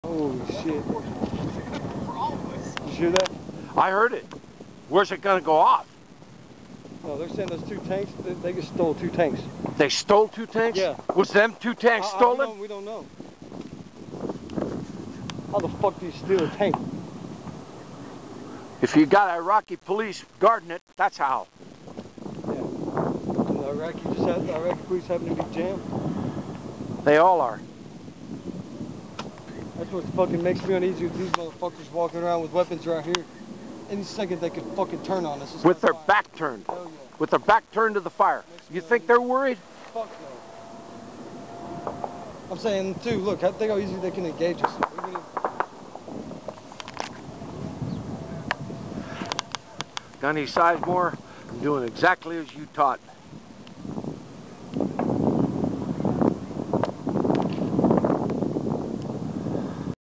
03 25 08 RT SILVER SADIR CITY, IRAQ "THEY STOLE TWO TANKS"
SGT PITTENGER  AND US SEE IRAQI TANKERS RUN FROM FIGHT